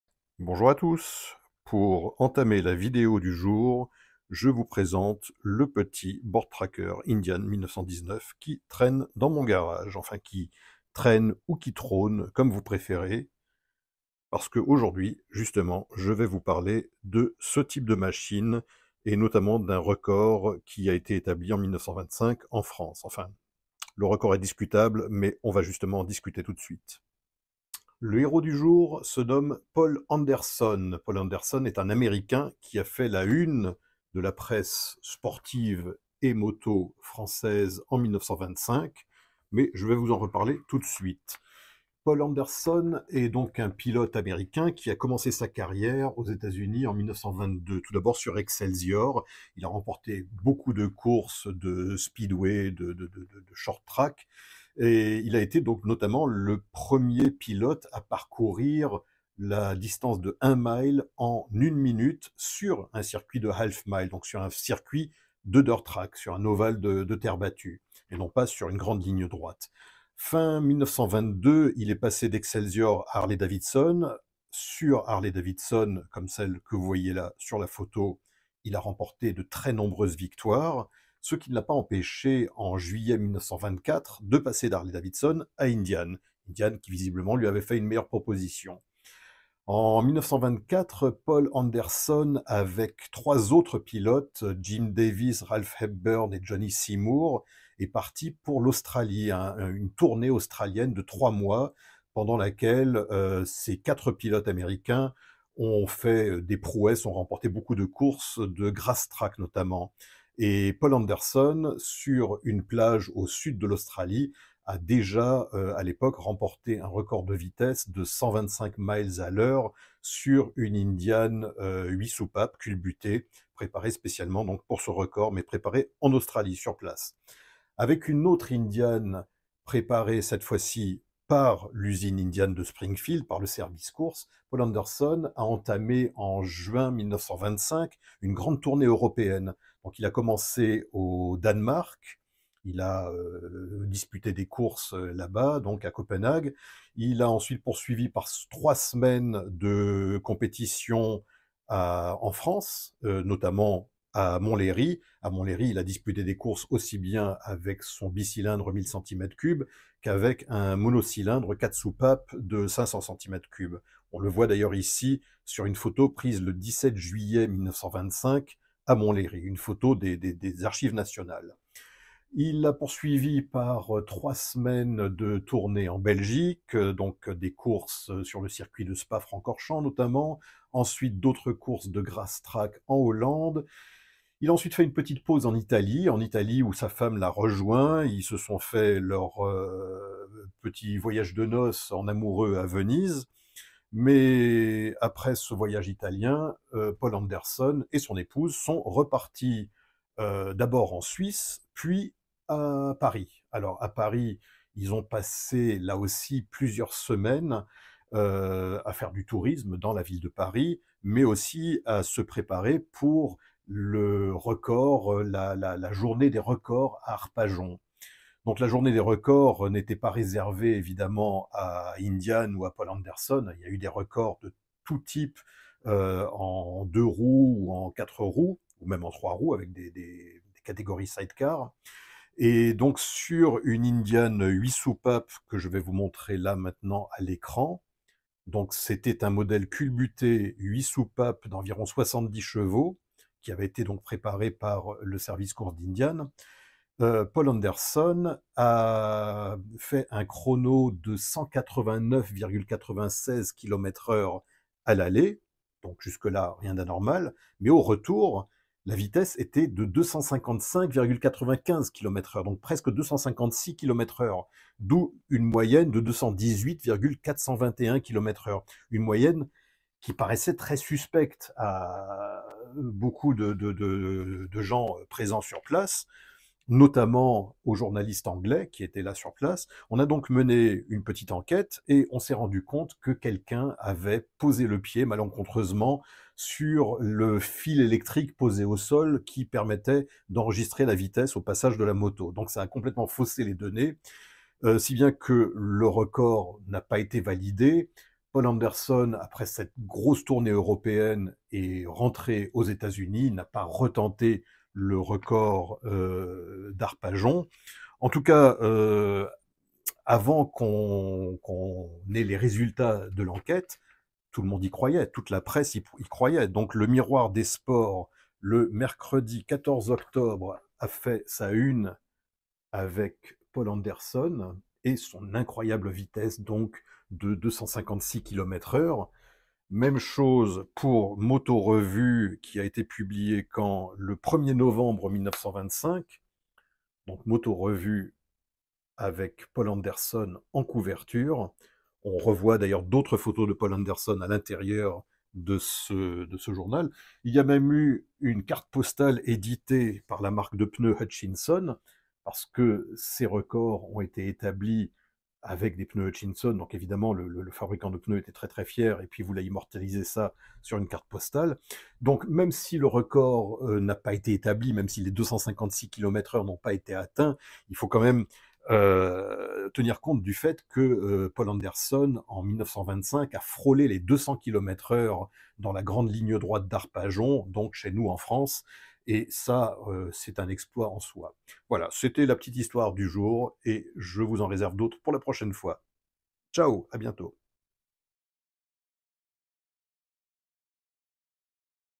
L'incroyable record du pilote Indian Paul Anderson à Arpajon en 1925...
En 1925, dans le cadre d'une grande tournée européenne, le pilote américain Paul Anderson a atteint une vitesse anormalement élevée sur son Indian 1000 cc à huit soupapes sur un tronçon de route à Arpajon, en France.